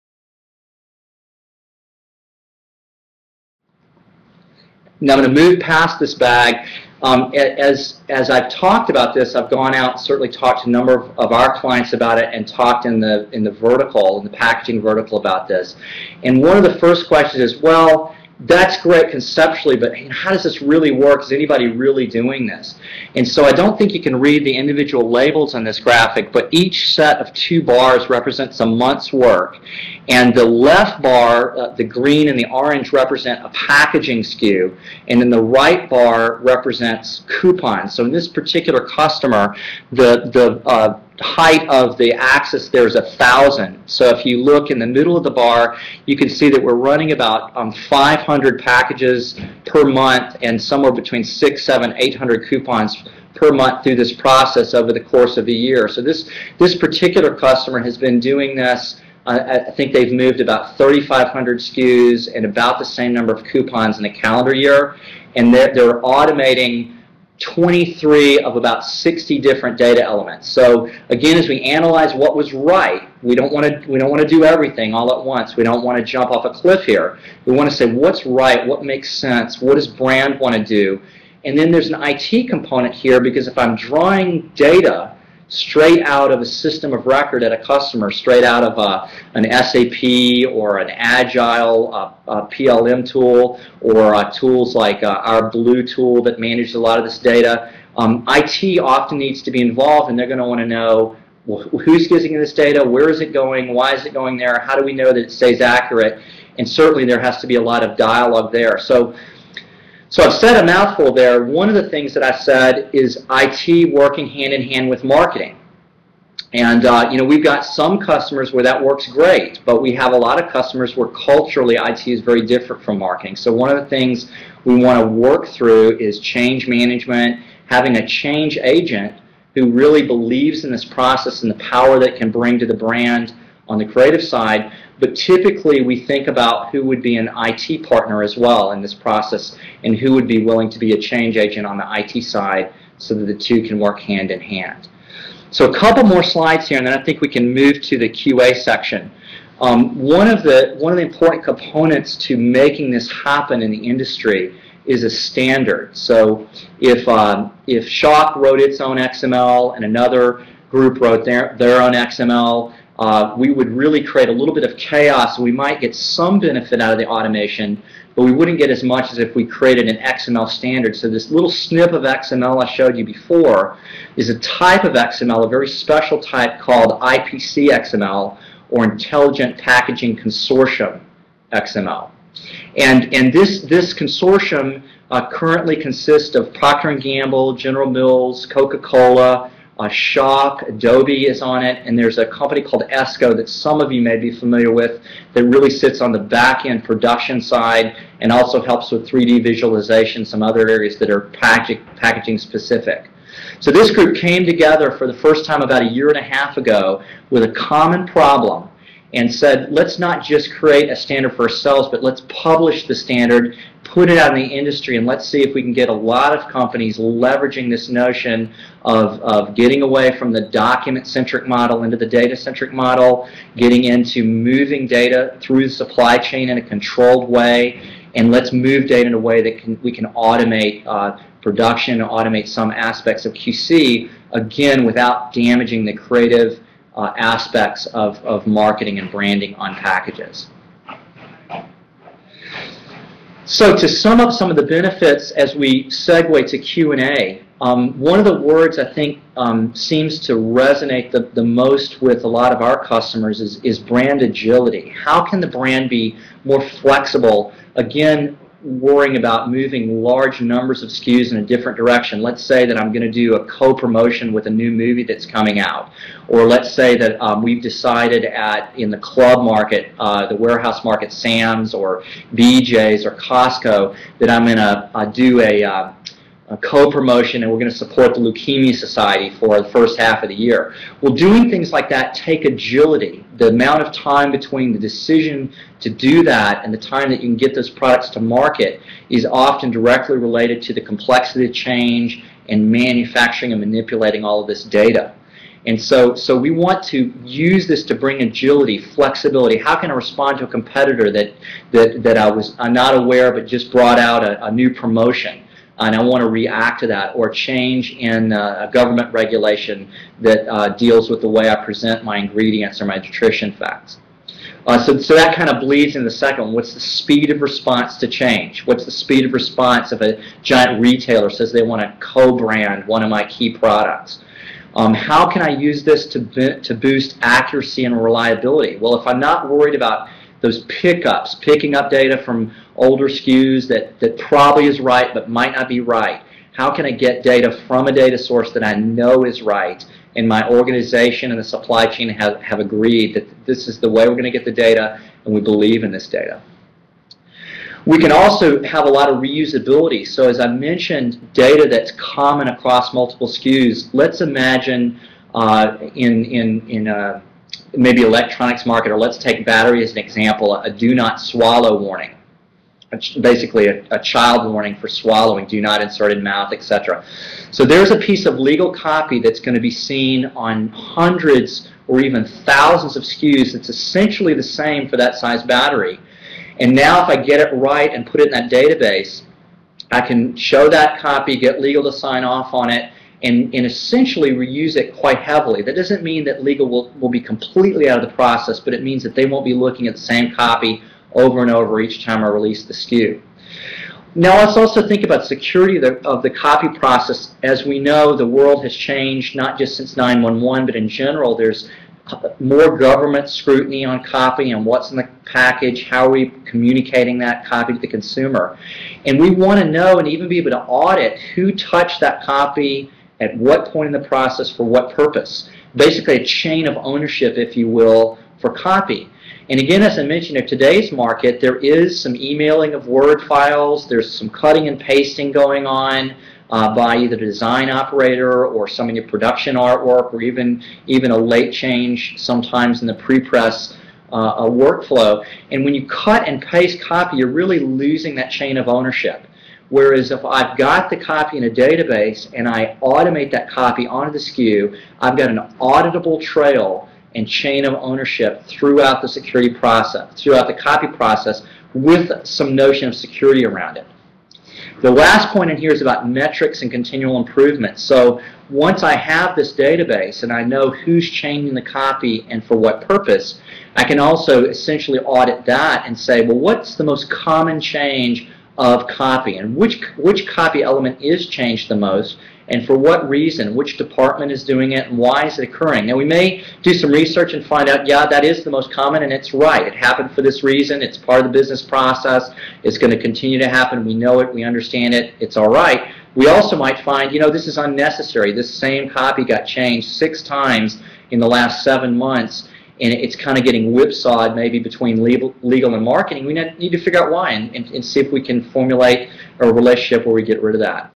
BrandSquare Live Session - Stephen Kaufman: The Intelligent Package Part III
Intelligent Packaging is an emerging trend in the packaging industry.  The live session took place on Schawk's online community, on May 12, 2009.  This video is part 3 of the session.